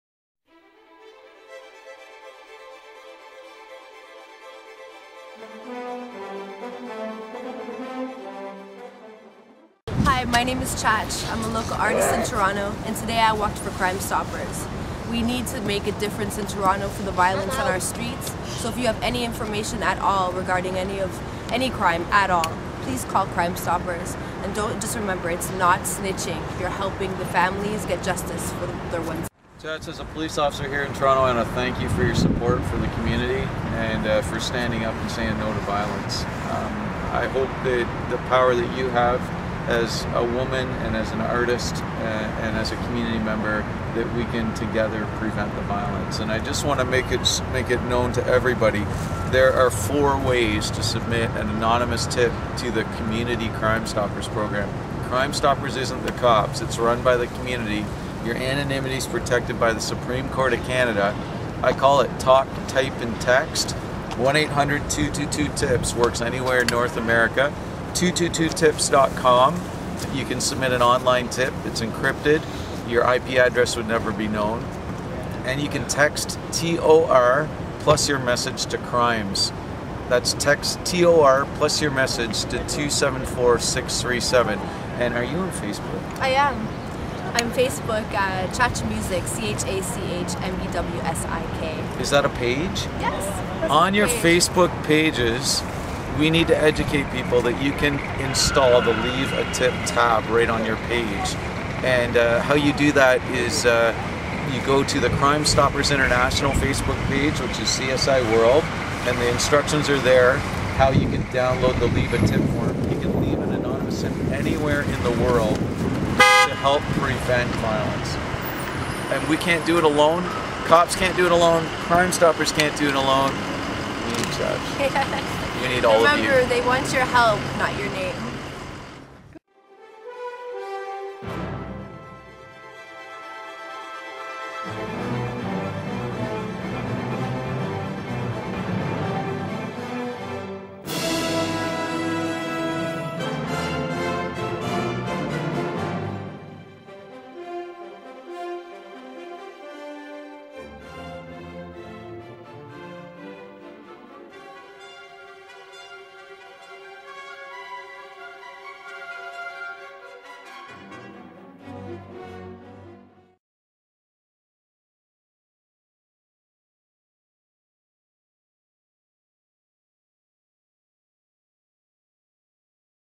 CANADIAN POP/DANCE/R&B STAR CHACH SUPPORTS TORONTO CRIMESTOPPERS
Chach joined Toronto Police and Crime Stoppers on October 24th 2010 to promote Crime Stoppers and support families at the "Walk for Murdered children in Toronto" and she is also planning to speak at a November 26th 2010 news conference of Toronto Musicians standing up against glorifying GUNS, DRUGS, GANGSTERISM AND DISRESPECTING WOMEN in their songs. Chach is very active in the community and wants to reach every young Torontonian, Canadian and all youngsters around the world.